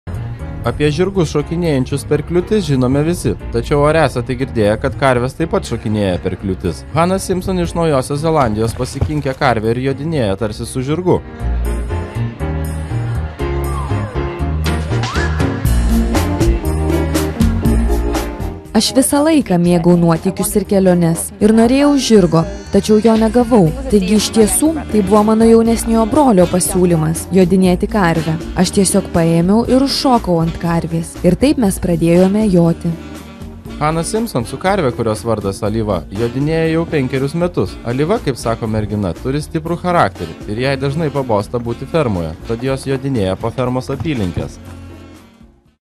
Jodinėjimui žirgas nebūtinas, ši mergina tai įrodė
Hana Simpson iš Naujosios Zelandijos visada norėjo žirgo, tačiau jo negavo, todėl pasikinkė karvę ir jodinėja ant jos. Mergina ant karvės vardu Alyva jodinėja jau penkerius metus.